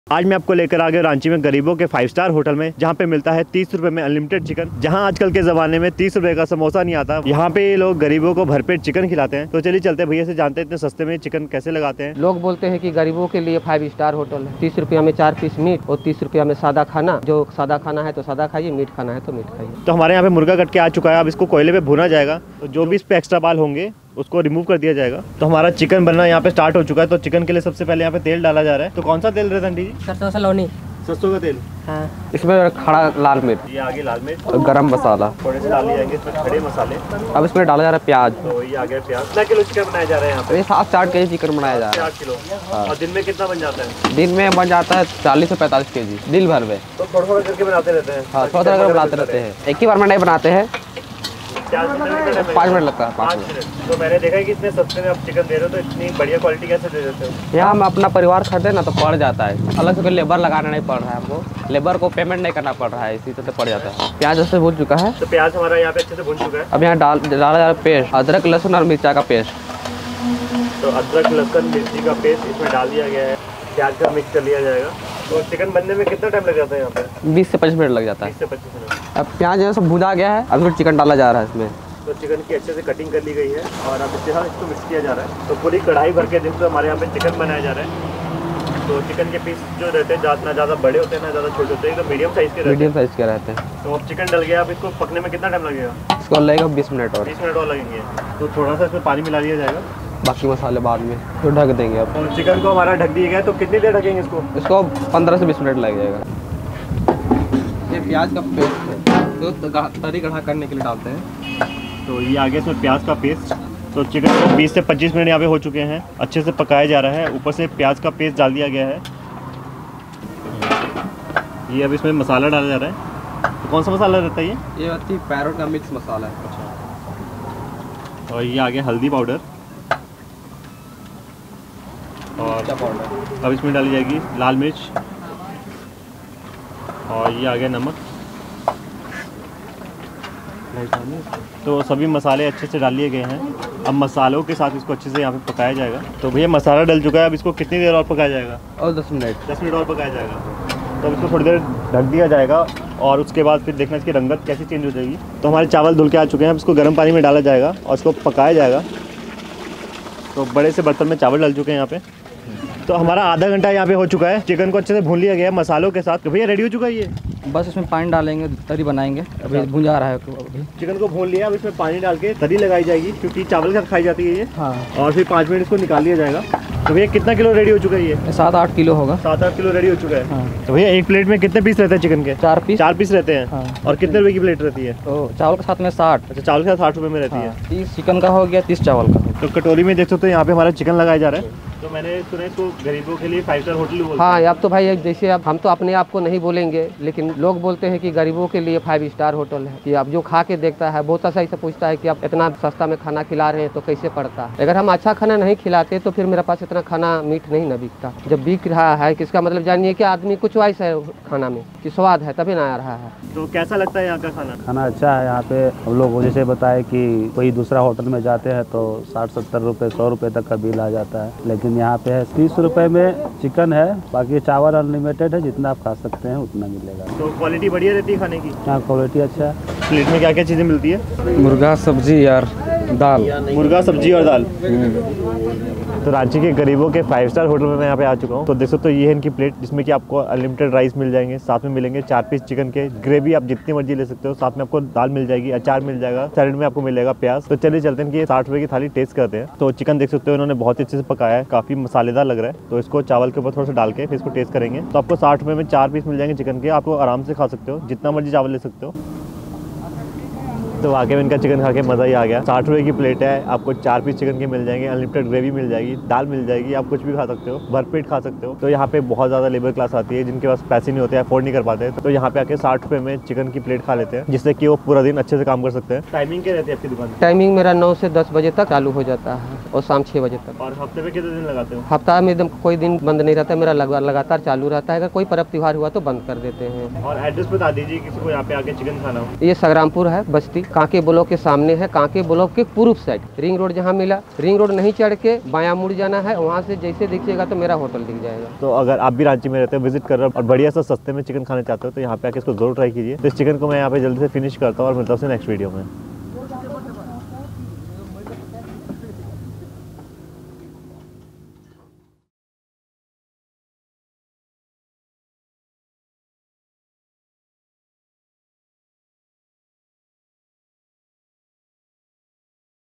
Gareebo Ka 5-Star Hotel Making Unlimited Chicken Rice In Ranchi Rs. 30/- Only l Ranchi Street Food
Address: Patratoli, Kanke Block, Ring Road, Ranchi, Jharkhand 834006